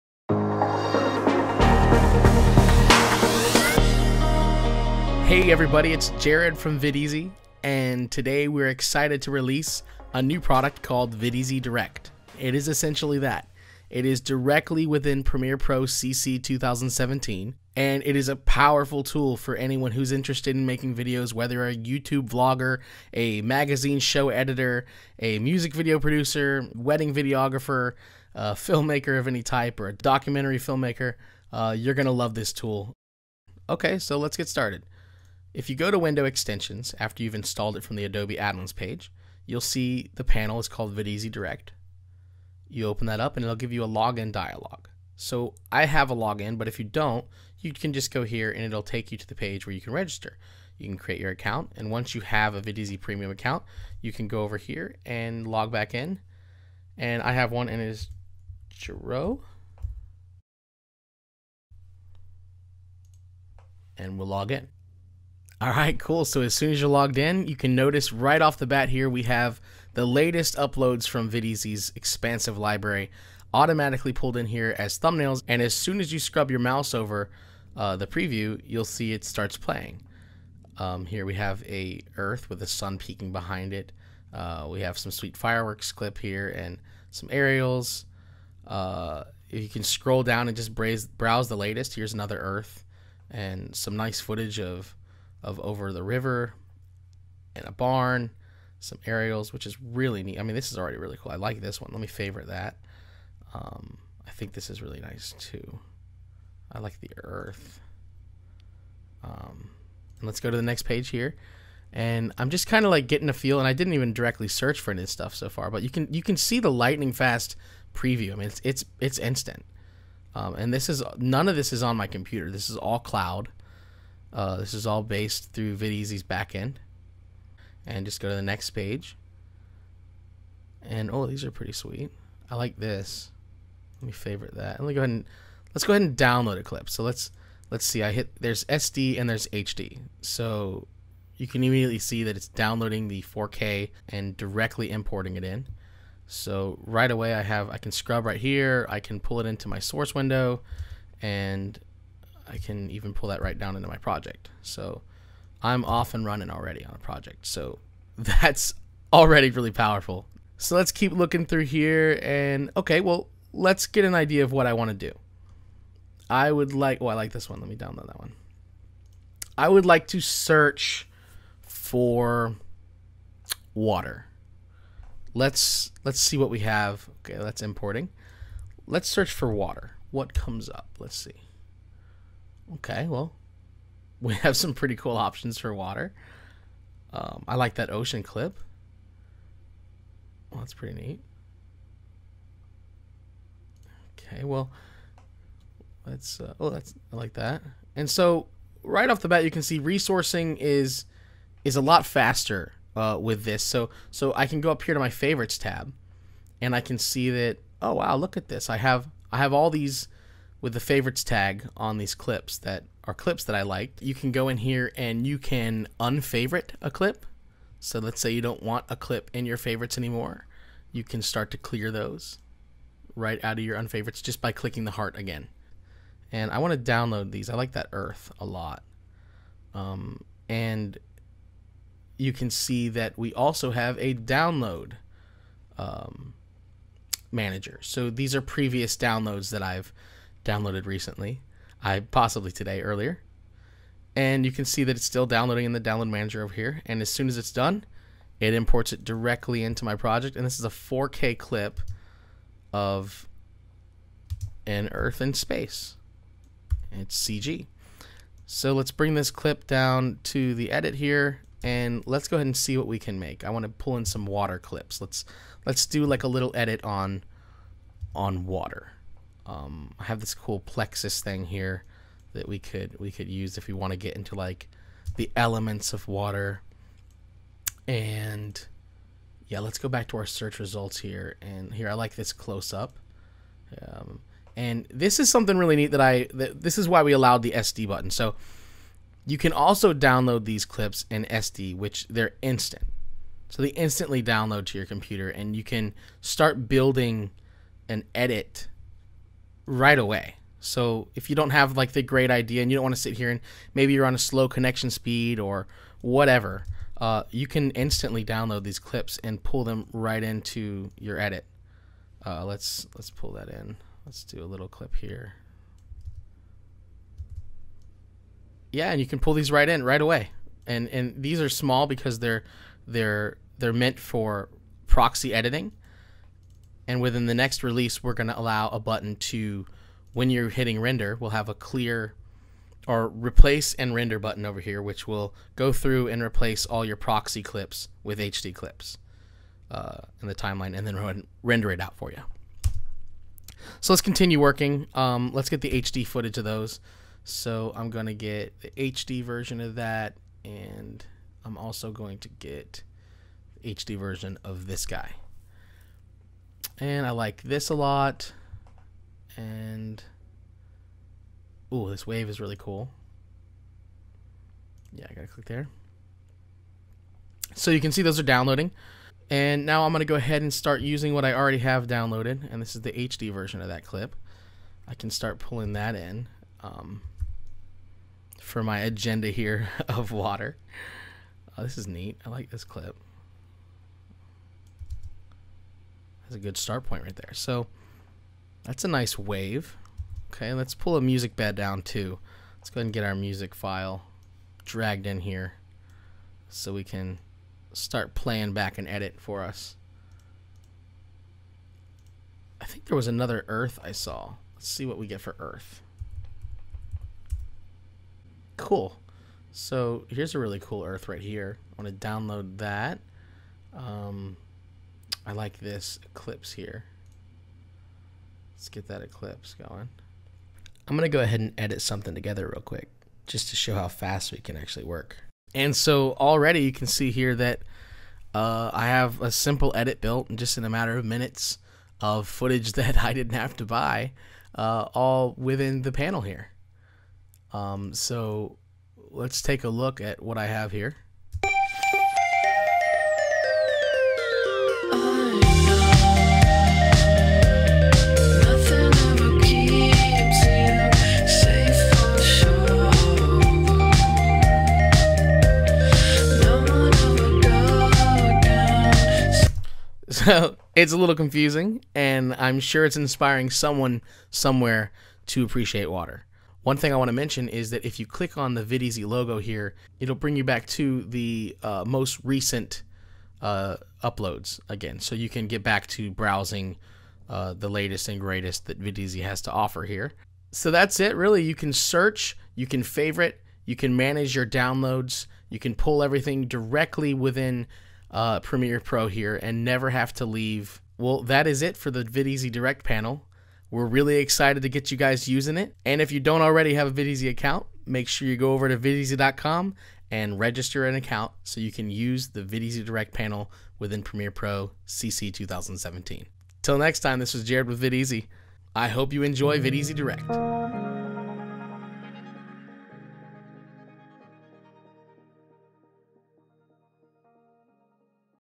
Introduction to Videezy Direct
Videezy Direct gives you the ability to resource and edit faster than ever before. All without leaving the Premiere Pro environment.

With lightning-fast previews you can search our expansive library of free HD & 4K stock footage, motion backgrounds, and video assets. Instantly import your free downloads directly into your Premiere Pro project. You'll finish your entire project without ever leaving the Premiere Pro.